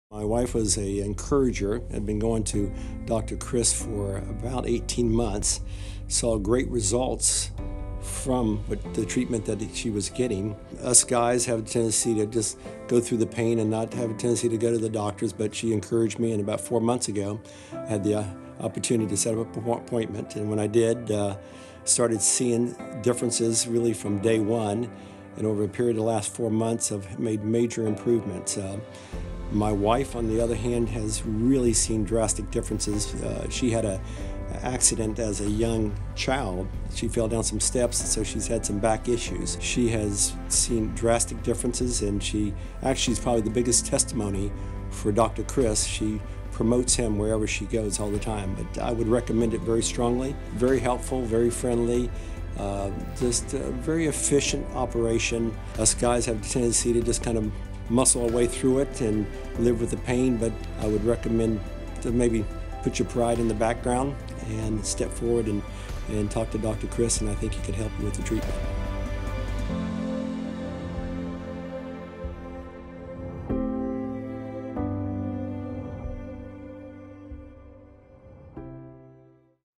Boosting Overall Health and Relieving Pain with Upper Cervical Care | MONTY'S STORY
This is Monty's Story:

After 18 month's of seeing his wife's health improve under Upper Cervical Care, Monty was convinced that it was effective.  Despite his tendency to avoid doctors, he decided that this would be a wise decision to relieve pain.  After his first adjustment, he immediately noticed differences in his health, so he fully recommends Upper Cervical Care as an efficient approach to healing.

You could have an experience like Monty's through Upper Cervical care.

St. Petersburg Office
2090 9th St. N.